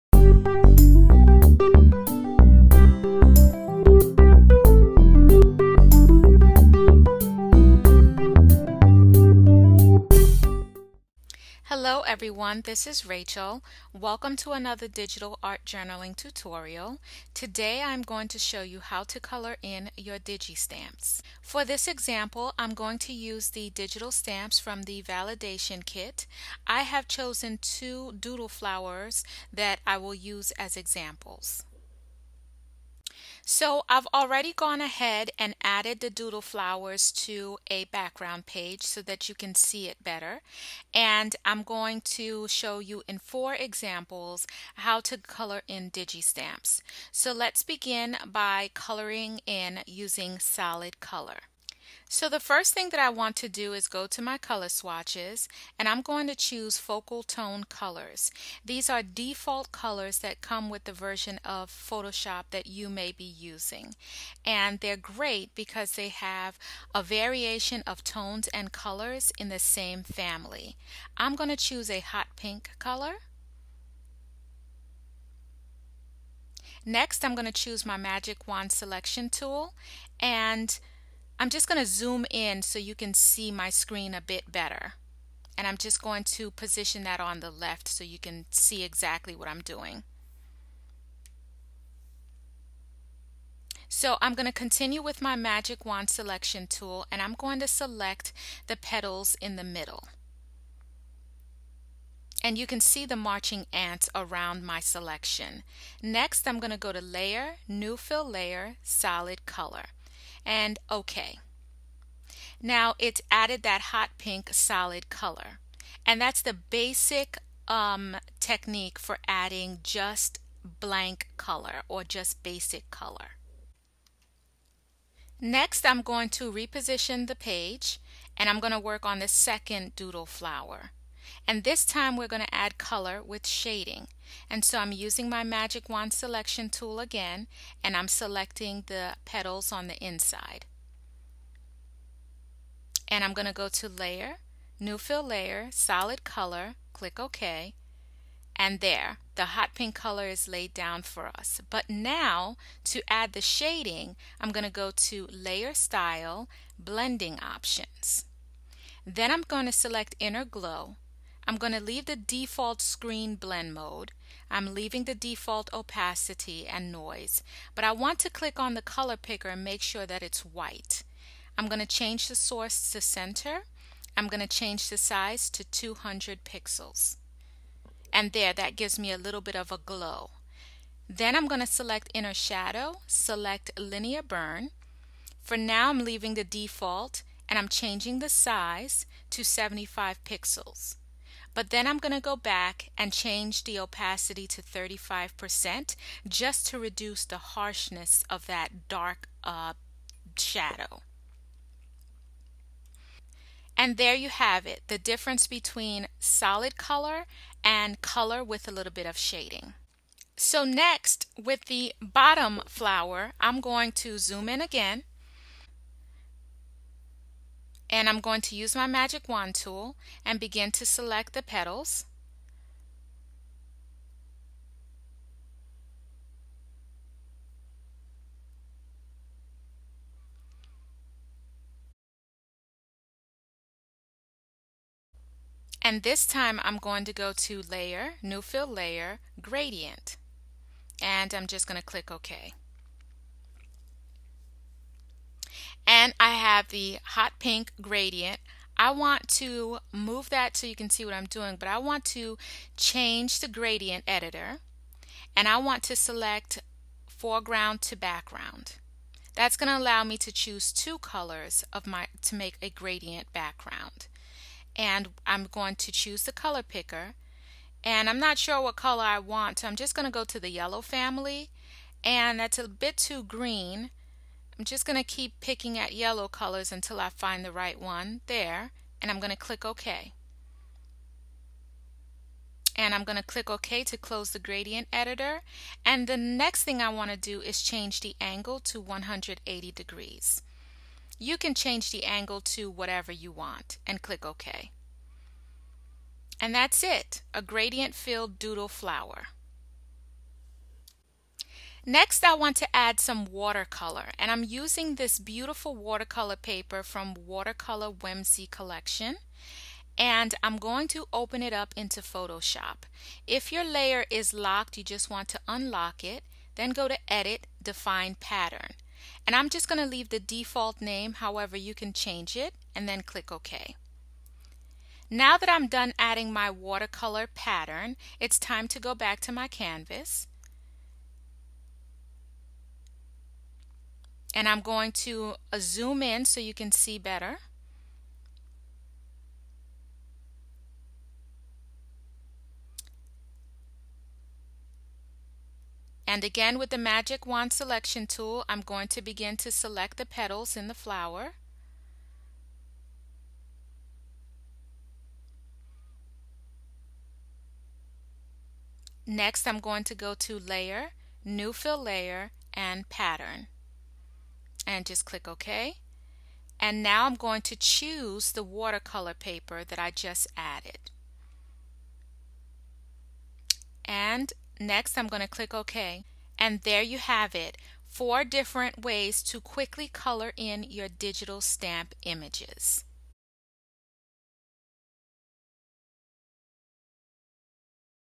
How To Color Digi Stamps Using Solid Color, Solid w/Shading, Gradients and Patterns
Digital mixed media art journal project showing how to color in digital stamps using the New Fill Layer - Solid, Gradients or Pattern options in Photoshop.